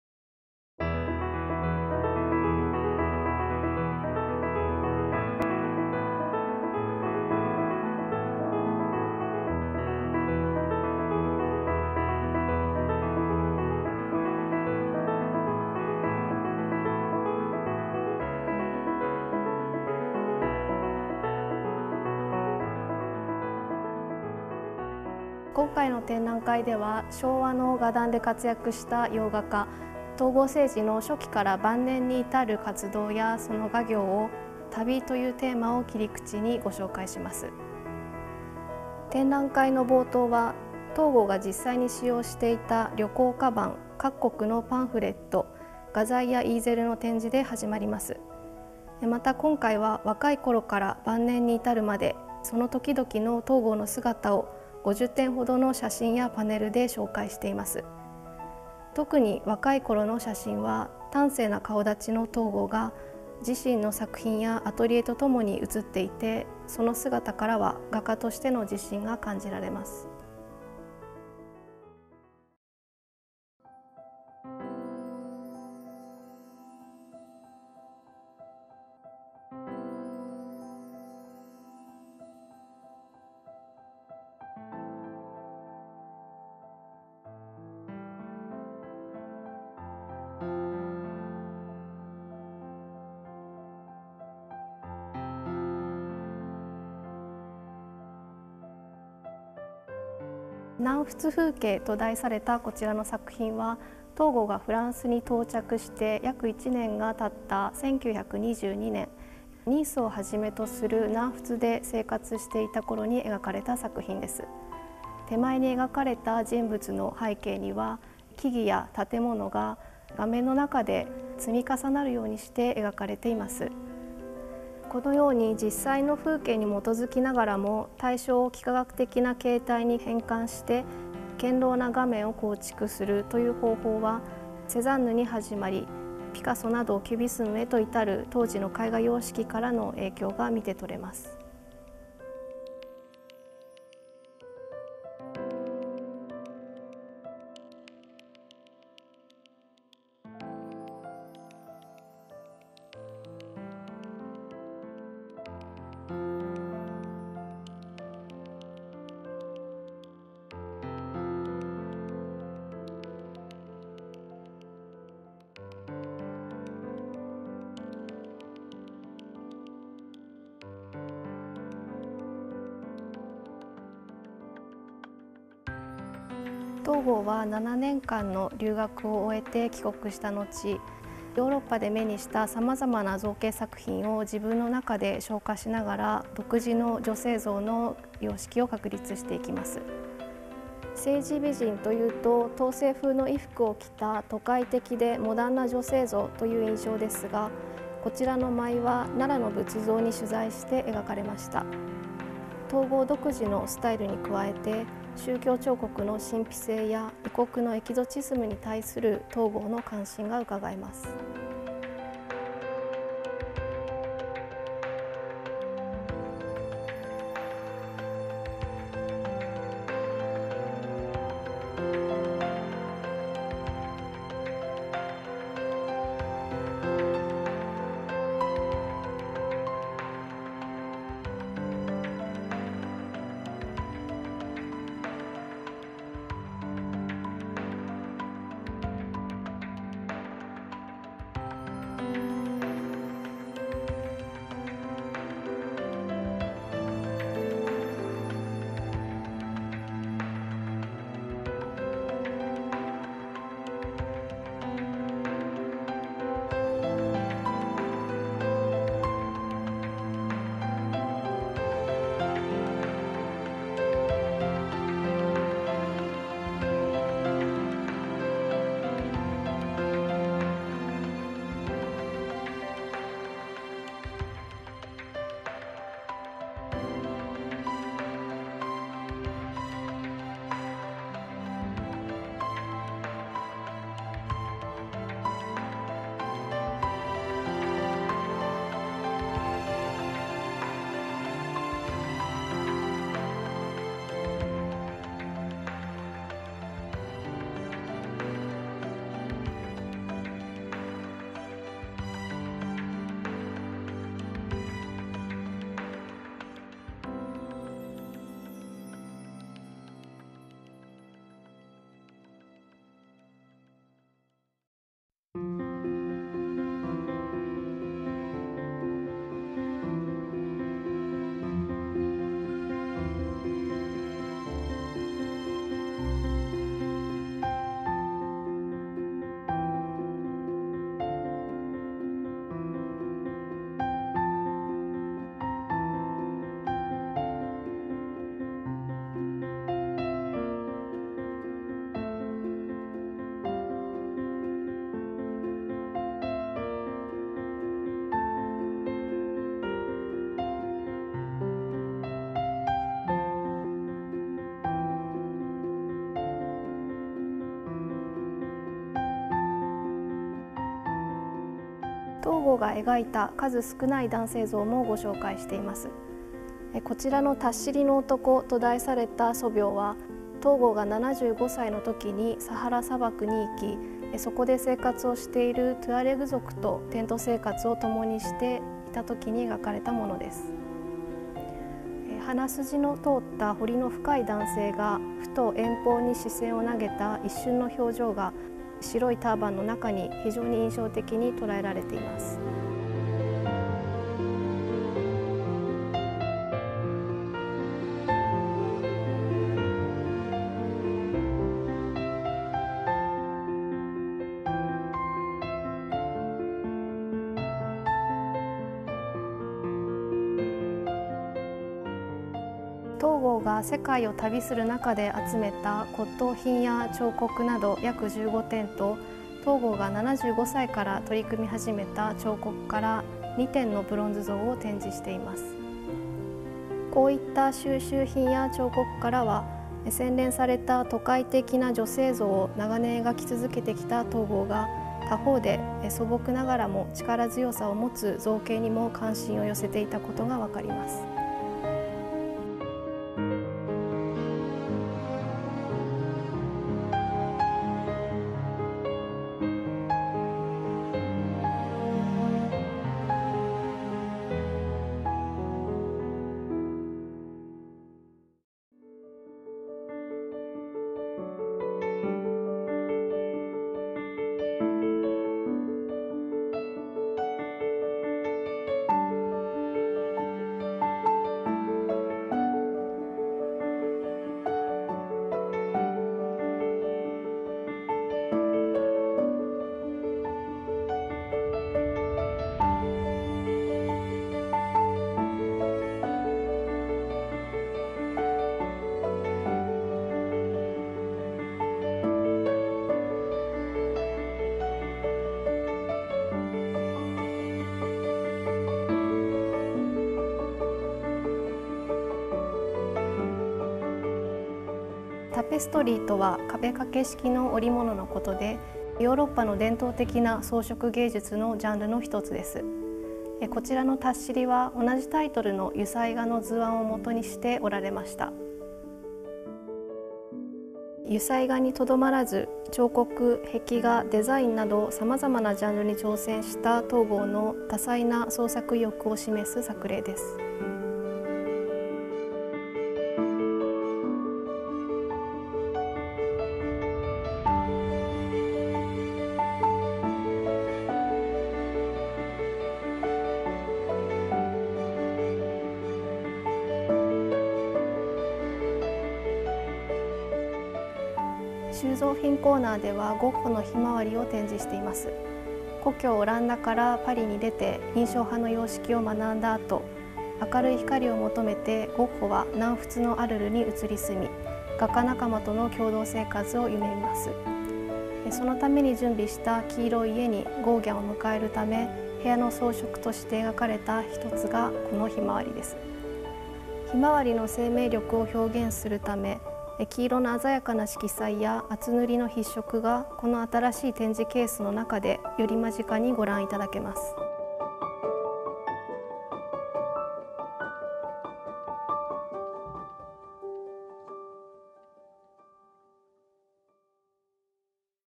「東郷青児 蔵出しコレクション～異国の旅と記憶～」紹介映像
「東郷青児 蔵出しコレクション～異国の旅と記憶～」（２０２０年１１月１１日～２０２１年１月２４日）の展覧会紹介映像です。